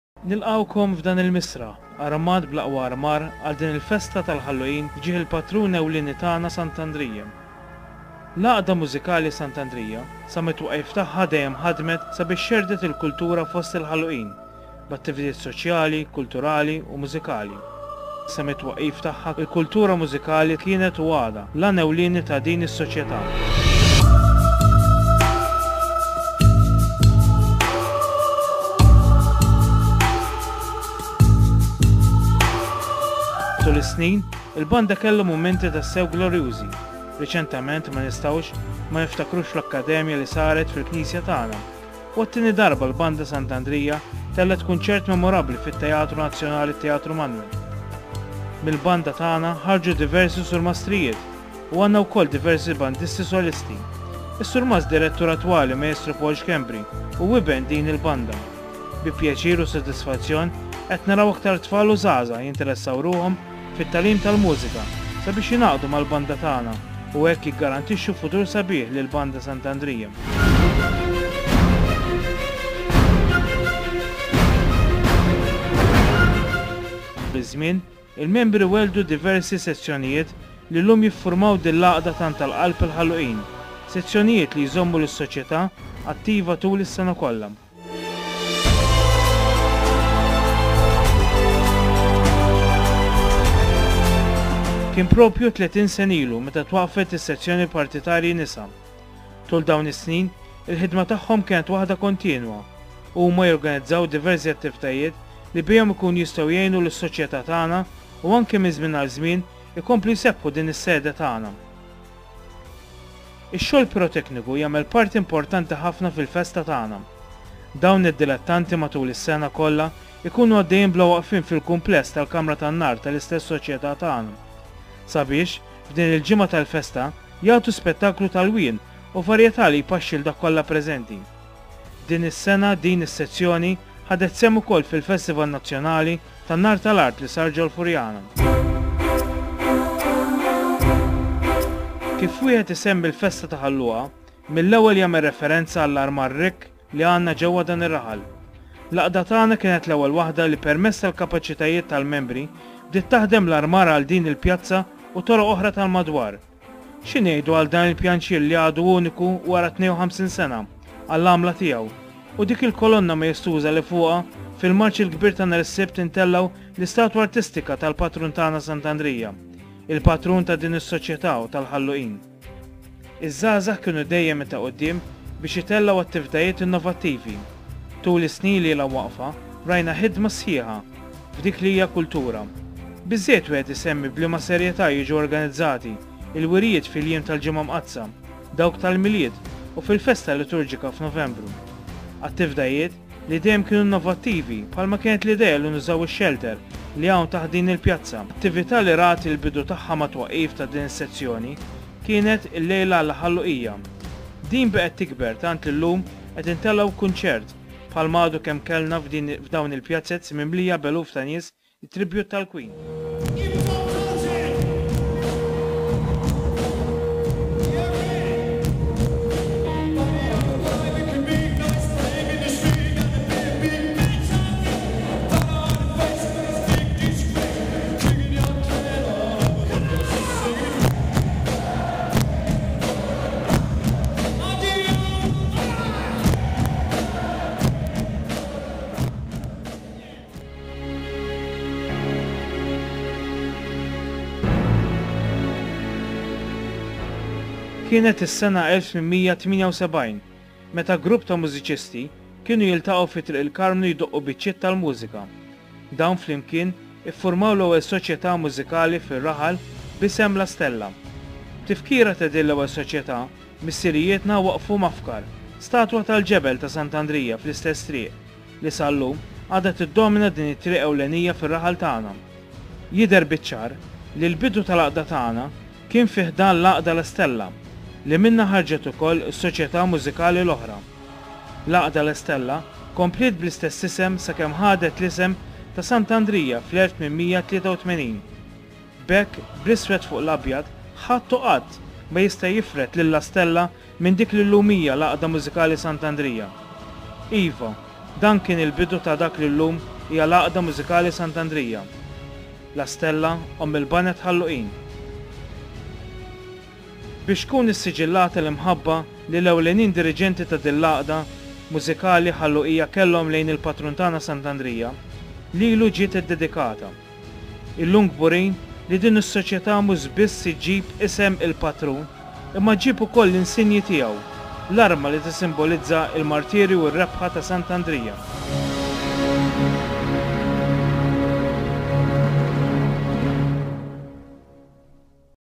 St Andrew's Band Club, Luqa feature - Festa 2011
Feature on St Andrew's Band Club of Luqa, Malta during the Feast held in JULY 2011.